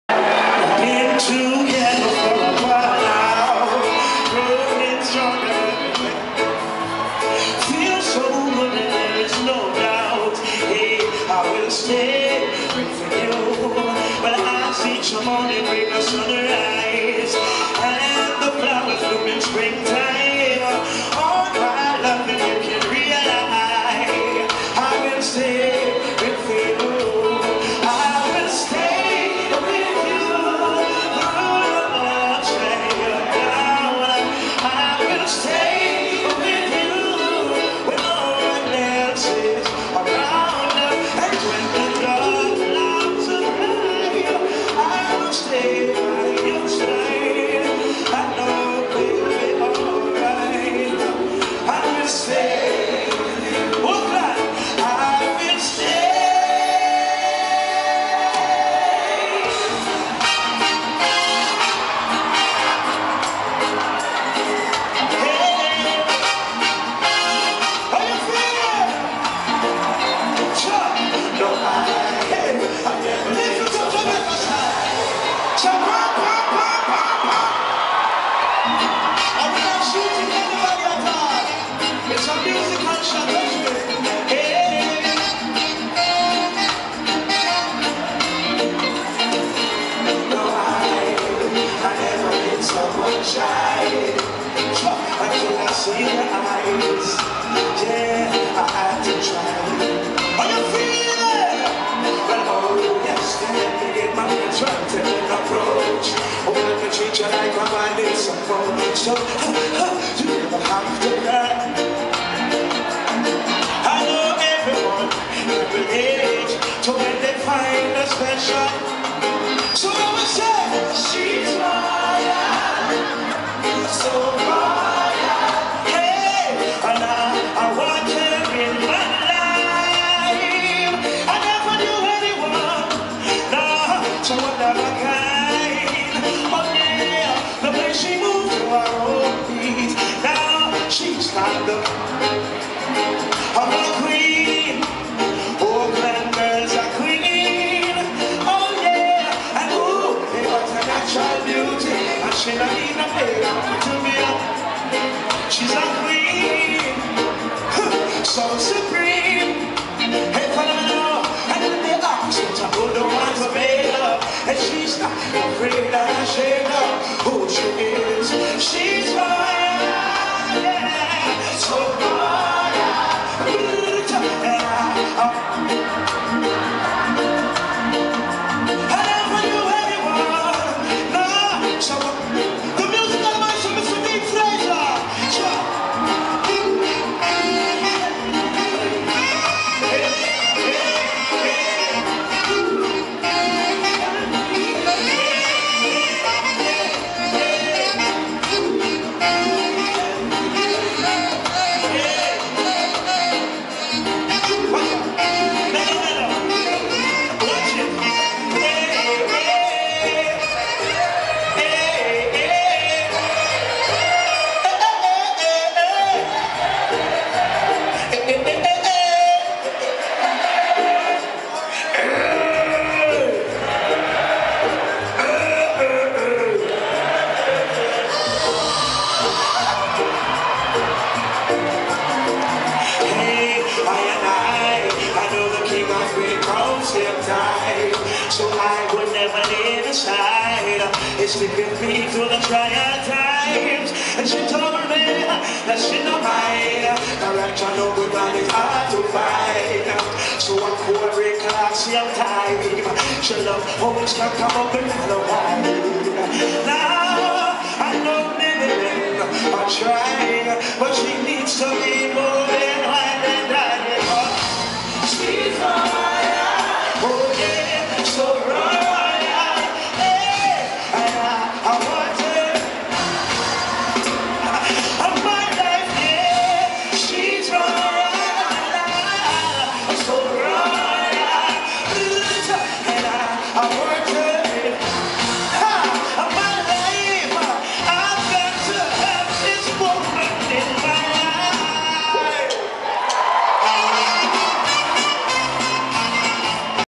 Tarrus Riley live at Fox Theater in Oakland, California Part VI
Tarrus Riley performing live at the Fox Theater in Oakland, California on Saturday, February 20, 2010 (Ragga Muffin Festival / Moss Jacobs Presents in association with Another Planet Entertainment)